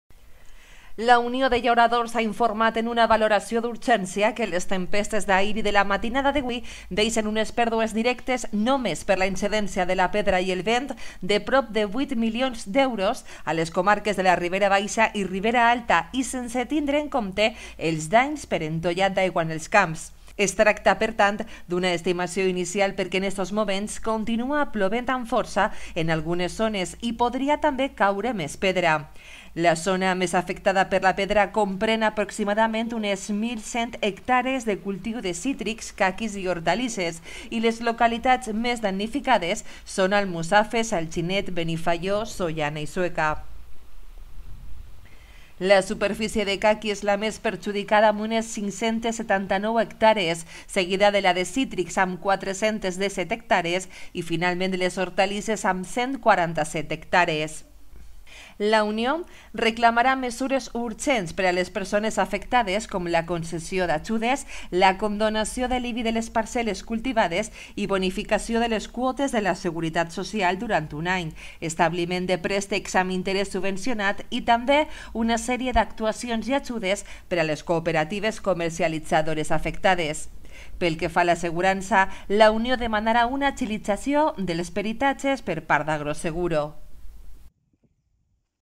El temporal deixa 8 milions en pèrdues en el camp
LA UNIÓ de Llauradors ha informat en una valoració d'urgència que les tempestes d'ahir i de la matinada de hui deixen unes pèrdues directes només per la incidència de la pedra i el vent de prop de 8 milions d'euros a les comarques de la Ribera Baixa i Ribera Alta, i sense tindre en compte els danys per entollat d'aigua en els camps.